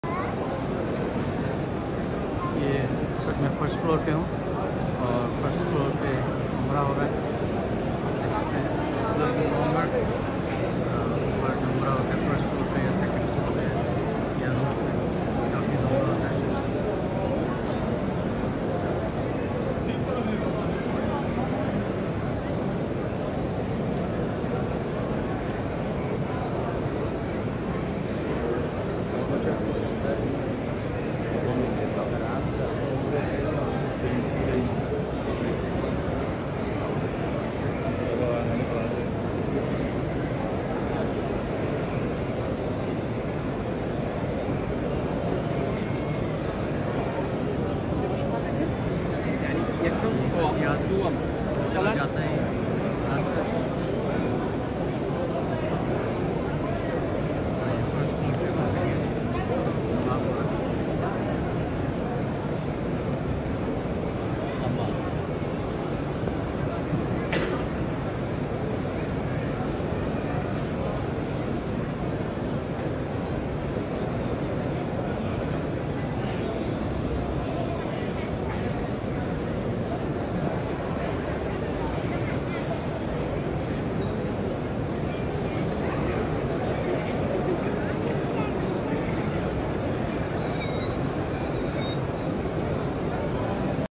People are performing Tawaf at first floor of Masjid Al Haram
The term ‘Tawaf’ is derived from the Arabic verb ‘Taafa’, which means ‘to encircle something’ or ‘to walk around something’. In the Islamic context, Tawaf refers to taking rounds or encircling the Holy Ka’abah seven times in an anti-clockwise direction as part of Umrah or Hajj, starting from Hajr-al-Aswad (the black stone). It’s among the most significant obligations of both Hajj and Umrah, without which the greater and minor pilgrimage would be incomplete. When performing Tawaf, pilgrims recite Takbir and various other supplications based on the Sunnah of the Prophet s.